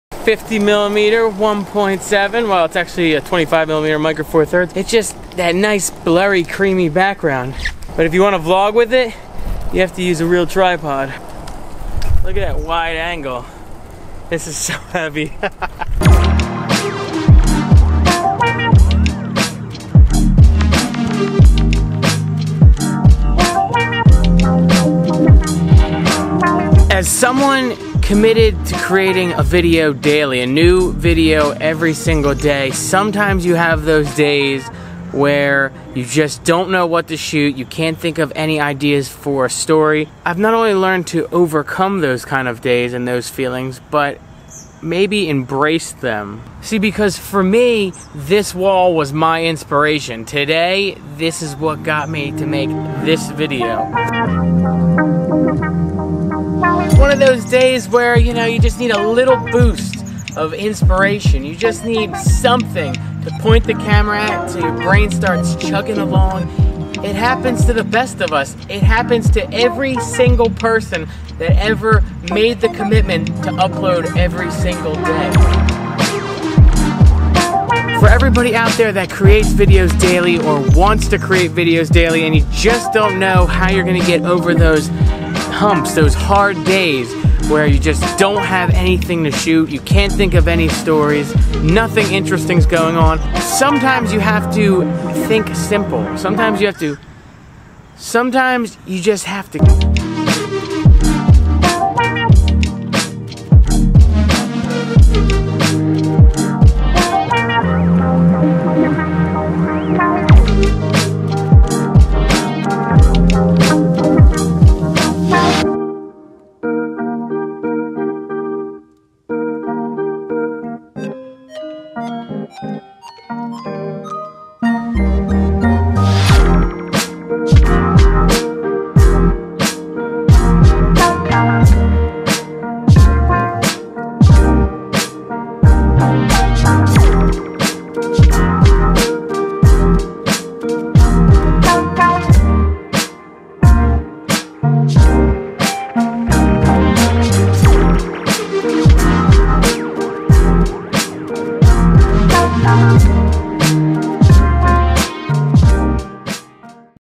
Want to UPLOAD DAILY but think you can't? YOU CAN
This is one of those days that would prevent most people from becoming a daily uploader, daily vlogger, daily whatever. Learn to embrace the boring days and you can conquer the world!

Alternate title: The Wall

SOCIAL:

SNAP: jonnyb-rad
TWIT: itsjonbradley